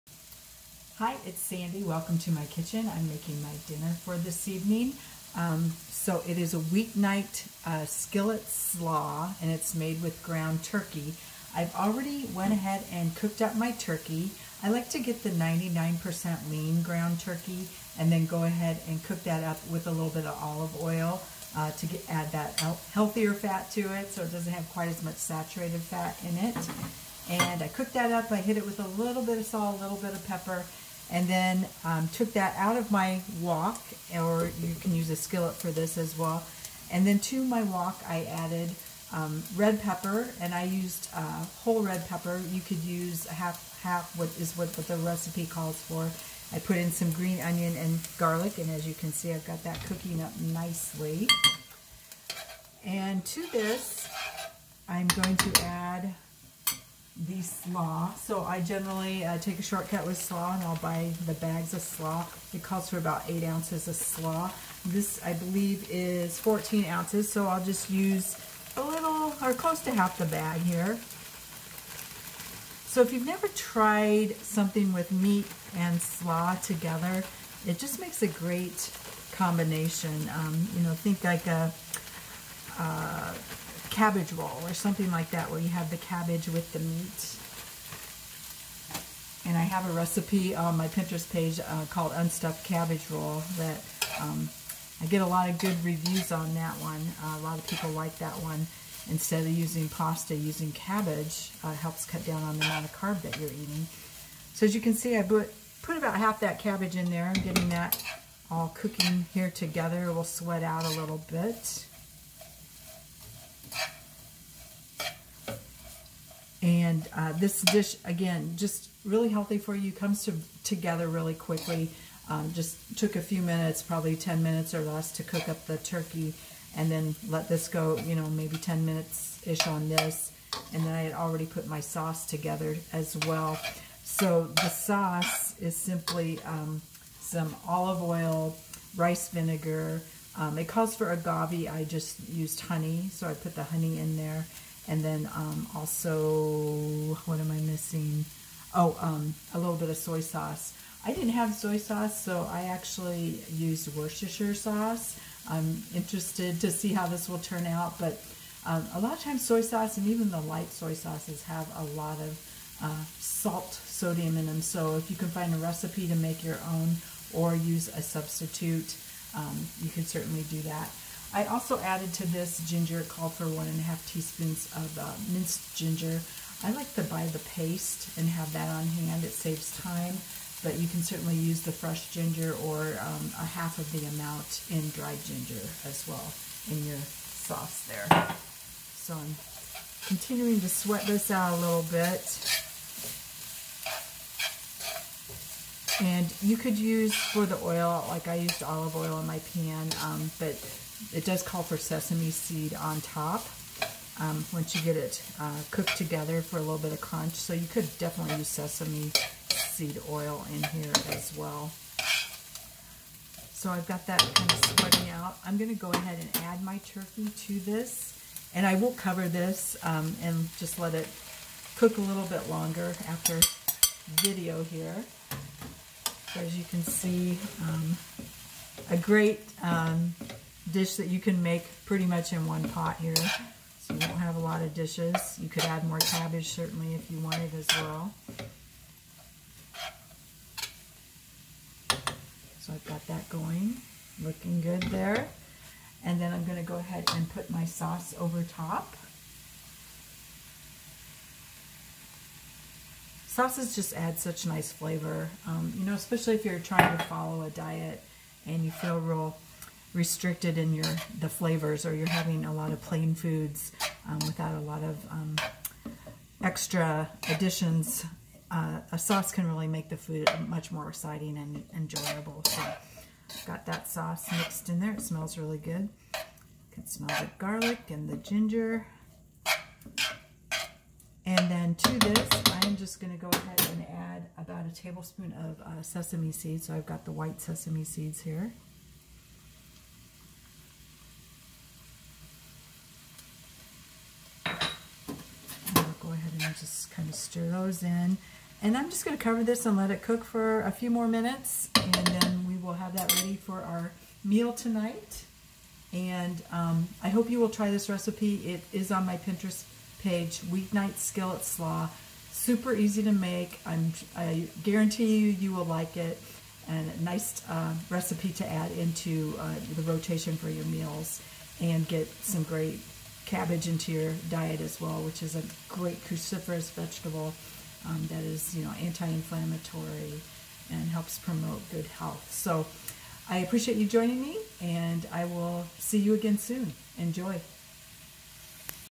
Weeknight Skillet Slaw
Make this delicious main dish featuring ground turkey, slaw, ginger and garlic.  With Sandy Holterhaus, RDN, LD, CYT.